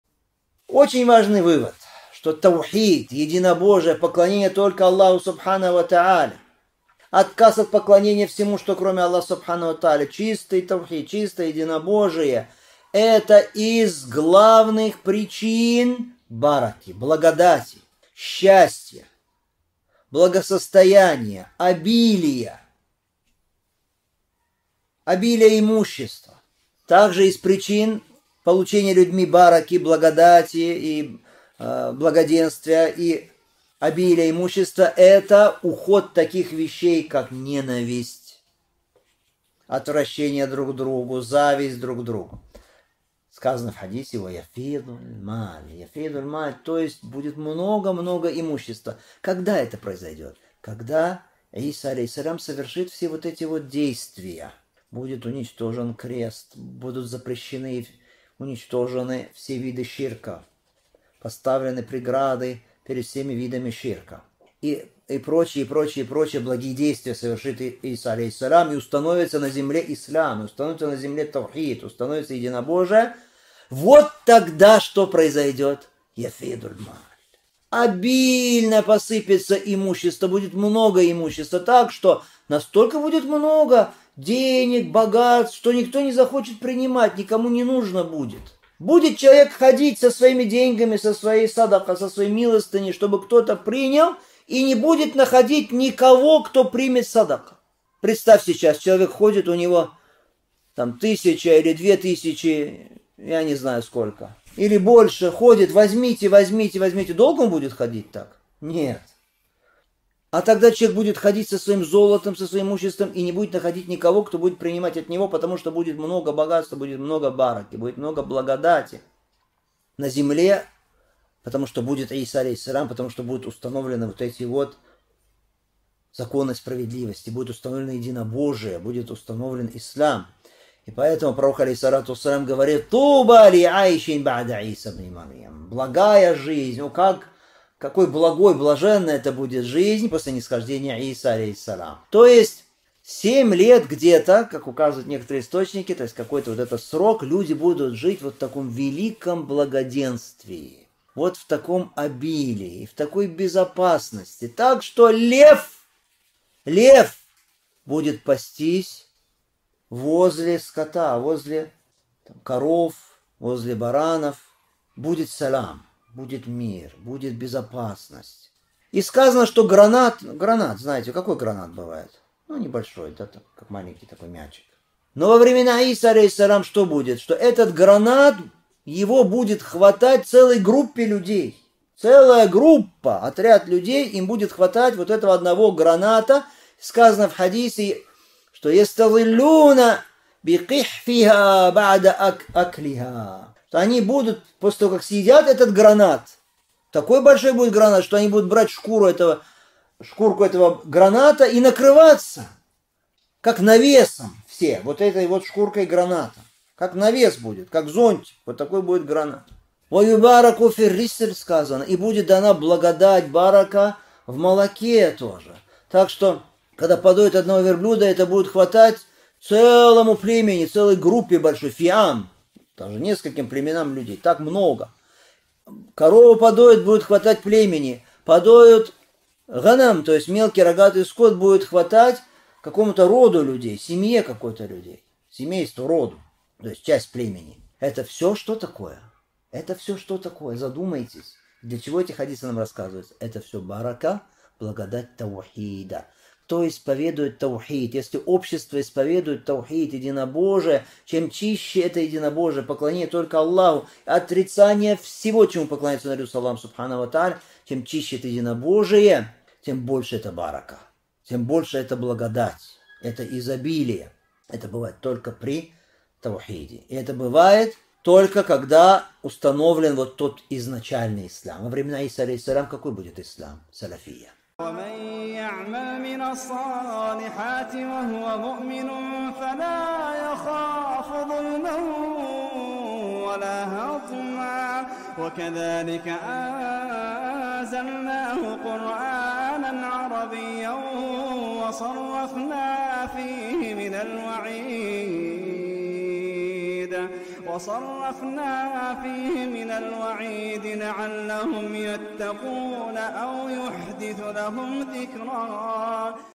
Единобожие — причина счастья и благополучия | Абу Яхья Крымский | Пользы из уроков по Сахиху Муслима
Целая лекция на Google Диск:
61. Сахих Муслим. Книга веры

Очень важный вывод, что Единобожие (Таухид) в поклонении только Аллаху (пречист Он и возвышен), отказ от поклонения всему, что кроме Аллаха (пречист Он и возвышен), чистое Единобожие (Таухид) – это из главных причин благодати (бараки), счастья, благосостояния, обилия имущества. Также из причин получения людьми благодати (бараки), благоденствия и обилия имущества – это уход таких вещей, как ненависть, отвращение друг к другу, зависть друг к другу. Сказано в хадисе:

ويفيضُ المالُ

«...,будет много-много имущества...» Этот хадис передали аль-Бухари (2222) и Муслим (155).

 Когда это произойдёт? Когда Иса (мир ему) совершит все вот эти вот действия: будет уничтожен крест, будут запрещены, уничтожены все виды многобожия (ширка), поставлены преграды перед всеми видами ширка и прочие благие действия, которые совершит Иса (мир ему), и установится на земле Ислам, Единобожие (Таухид).

Вот тогда обильно посыплется имущество, будет много имущества, настолько будет много денег, богатств, что никто не захочет принимать, никому не нужно будет. Будет человек ходить со своими деньгами, со своей милостыней (съадакъой), чтобы кто-то принял, и не будет находить никого, кто примет съадакъу. Представьте: сейчас человек, у которого тысяча, две тысячи или больше, ходит: «Возьмите, возьмите». Долго ли он будет ходить так? Нет. А тогда человек будет ходить со своим золотом, со своим имуществом и не будет находить никого, кто будет принимать от него, потому что будет много богатства, много благодати (бараки) на земле, потому что будет Иса (мир ему), потому что будут установлены вот эти законы справедливости, будет установлено Единобожие, будет установлен Ислам.

Поэтому Пророк (да благословит его Аллах и приветствует) говорит:

 طُوبَى لعيشٍ بعْدَ المسيحِ

«Какой блаженной будет жизнь после (нисхождения) Исы (мир ему)!» Этот хадис передал ад-Дайлями в «аль-Фирдаус» (3943), шейх аль-Альбани назвал его достоверным.

То есть семь лет где-то, какой-то этот срок, как указывают некоторые источники, люди будут жить в таком великом благоденствии, таком обилии, такой безопасности, так, что лев будет пастись возле скота: возле коров, возле баранов. Будет мир (салям), будет безопасность.

И сказано, что во времена Исы (мир ему) будет хватать одного граната, который сейчас размером с небольшой, маленький мячик, целой группе, отряду людей. Сказано в хадисе, что он будет таким большим, что после того, как они съедят этот гранат,  они будут брать шкурку этого граната

ويستظلُّونَ بقِحفِها

и накрываться (все, как навесом, как зонтиком),

ويبارِكُ اللَّهُ في الرِّسْلِ حتَّى إنَّ اللِّقحةَ منَ الإبلِ تَكْفي الفِئامَ منَ النَّاسِ، واللِّقحةَ منَ البقرِ تَكْفي القبيلةَ، واللِّقحةَ منَ الغنمِ تَكْفي الفخِذَ، فبينما هم كذلِكَ

и будет дана благодать (баракя) в молоке, так что, когда подоят одну верблюдицу, этого будет хватать нескольким племенам (фиам) людей; корову подоят – будет хватать племени; подоят мелкий рогатый скот (гъанам) – будет хватать роду, семье (части племени)». Этот хадис передал Муслим (2937).

Задумайтесь, для чего нам рассказываются эти хадисы. Это всё благодать (баракя) Таухида. Если общество исповедует Единобожие (Таухид), чем чище это Единобожие (поклонение только Аллаху, отрицание всего, чему поклоняются наряду с Аллахом), тем больше эта благодать (баракя), это изобилие. Это бывает только при Таухиде. Это бывает, только когда установлен вот тот изначальный Ислам. Во время Исы (мир ему) будет саляфия.